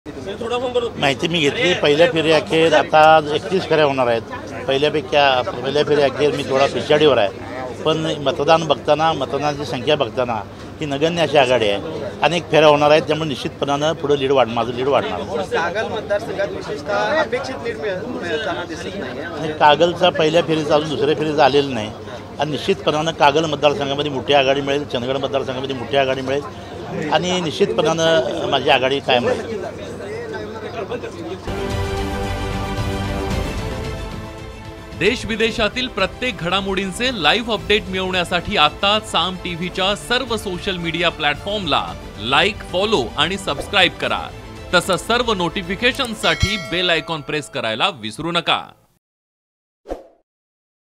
Sanjay Mandlik News | kolhapur लोकसभा मतदारसंघातून संजय मंडलिक पिछाडीवर..
Sanjay Mandlik News | kolhapur लोकसभा मतदारसंघातून संजय मंडलिक पिछाडीवर..| PRD